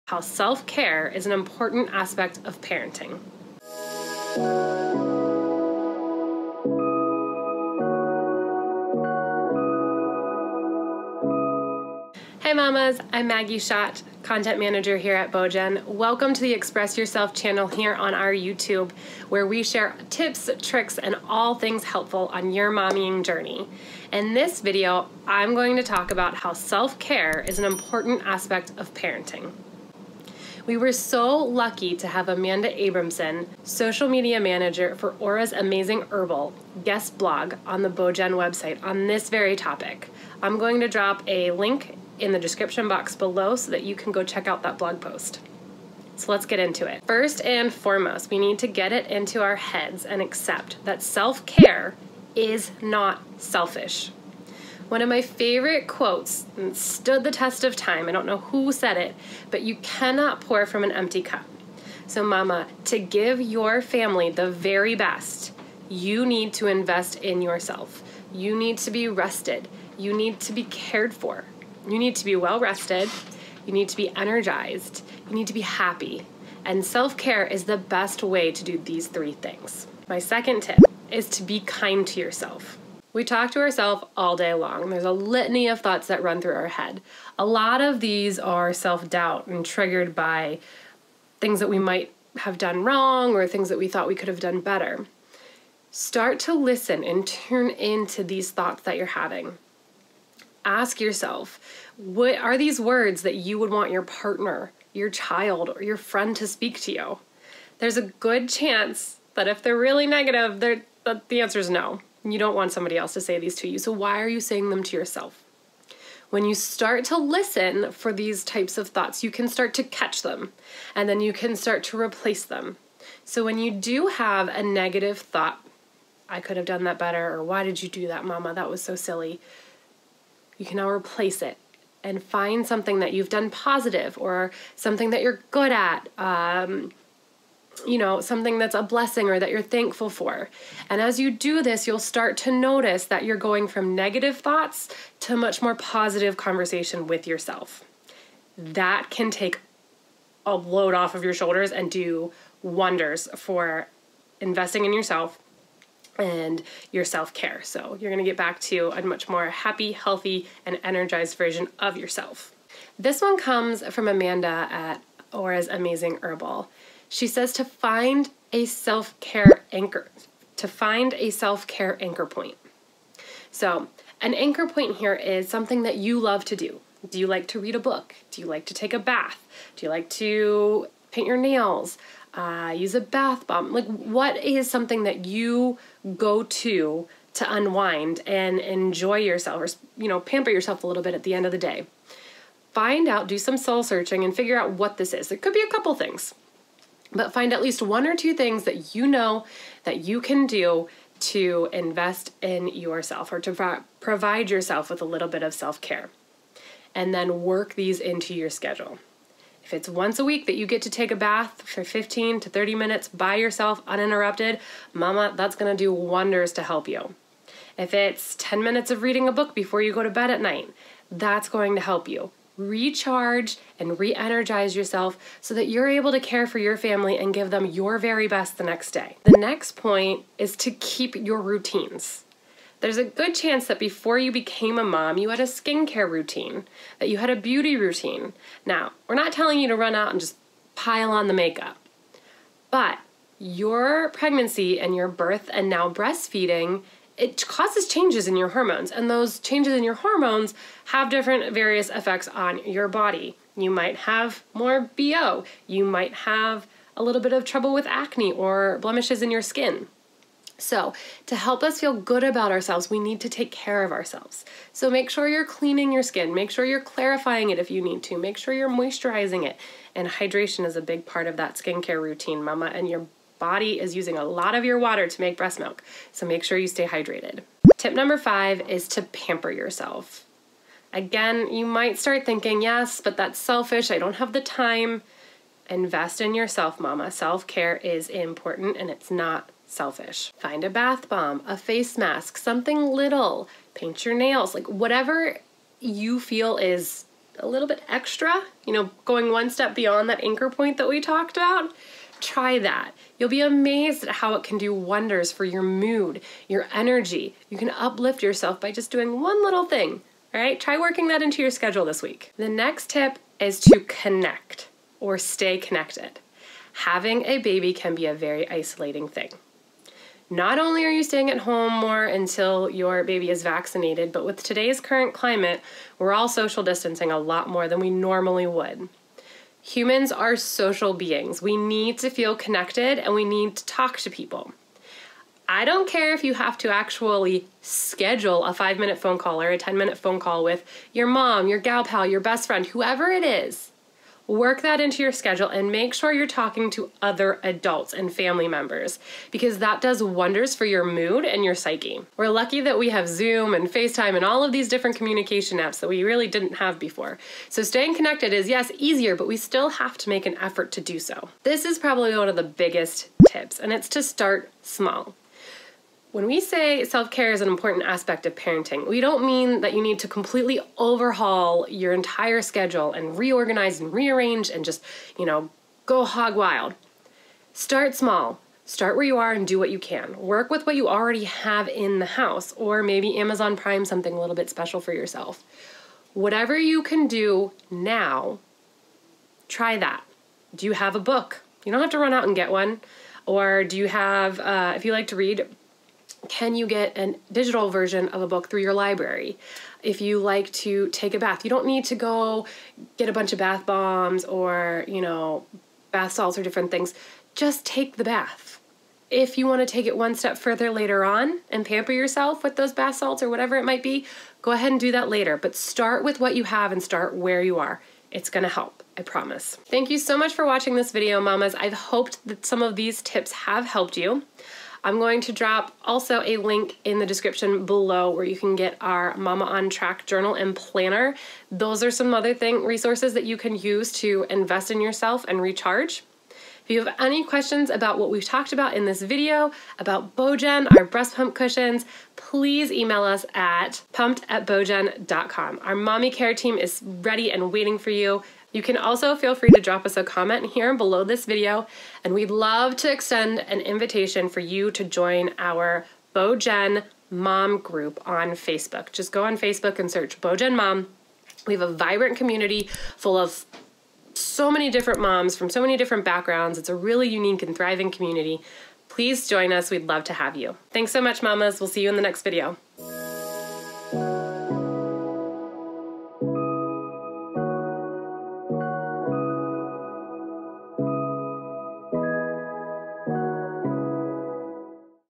Self Care is an Important Part of Parenting
Welcome to the Express Yourself video from BeauGen. In this video Maggie Schott explains why self care is an important part of parenting and provides helpful tips to incorporate it into your routine.

Ready to try or replace your Breast Pump Cushions?

Get to know BeauGen!